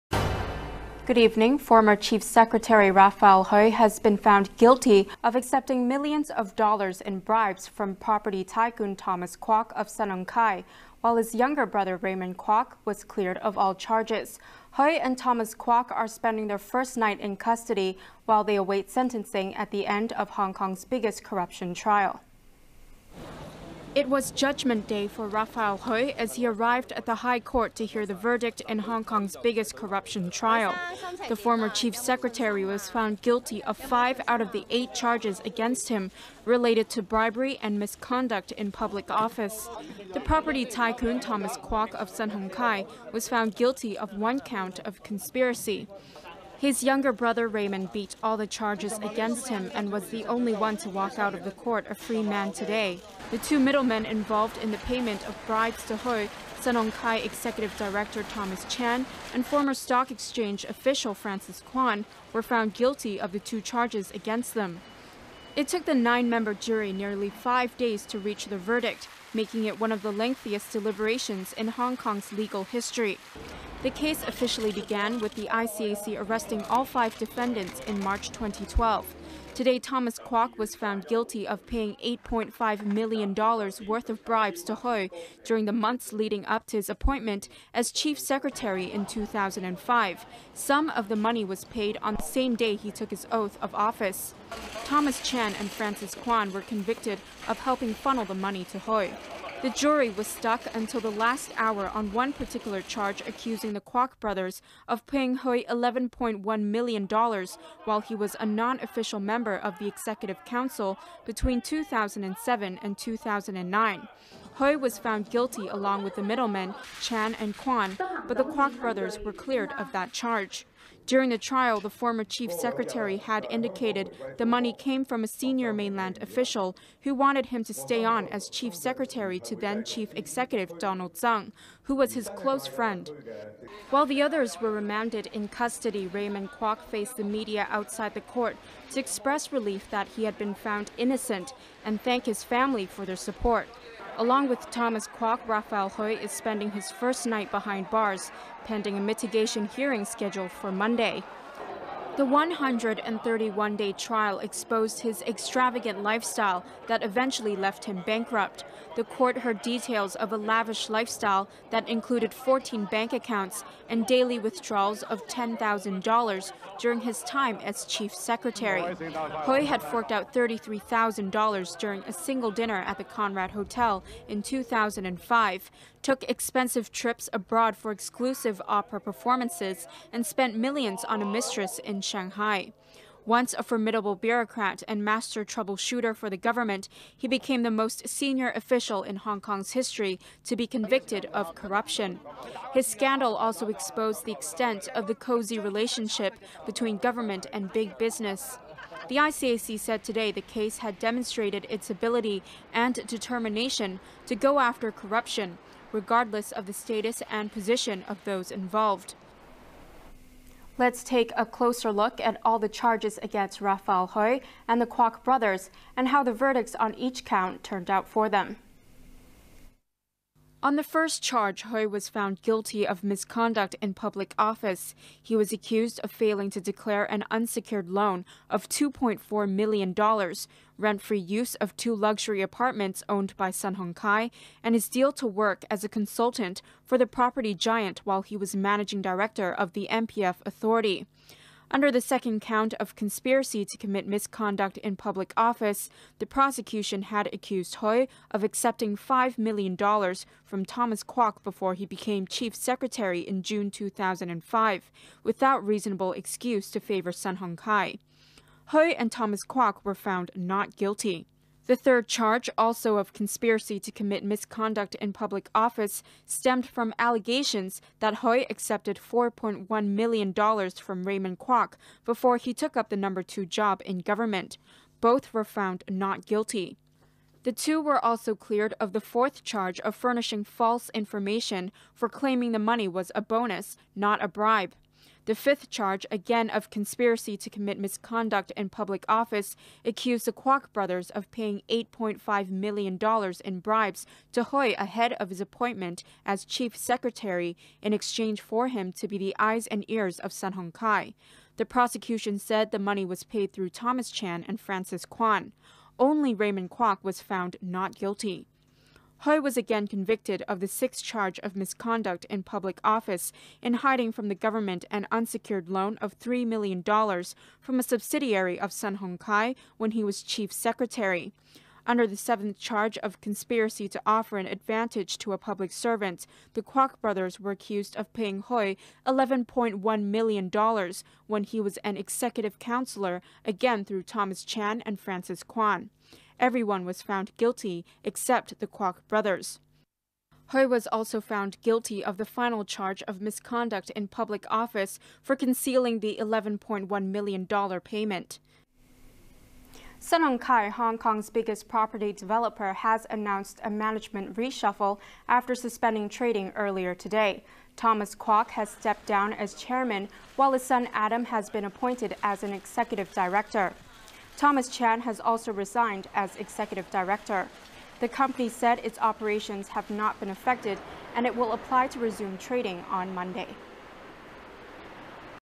Rafael Hui, Thomas Kwok, Guilty of Bribery
Former Chief Secretary Rafael Hui has been found guilty of accepting millions of dollars in bribes from property tycoon Thomas Kwok of Sun Hung Kai. Hui was also convicted on several counts of bribery and misconduct in public office, but Thomas Kwok's brother Raymond was cleared of all charges. Hui and Thomas Kwok are spending their first night in custody while they await sentencing at the end of Hong Kong's biggest corruption trial.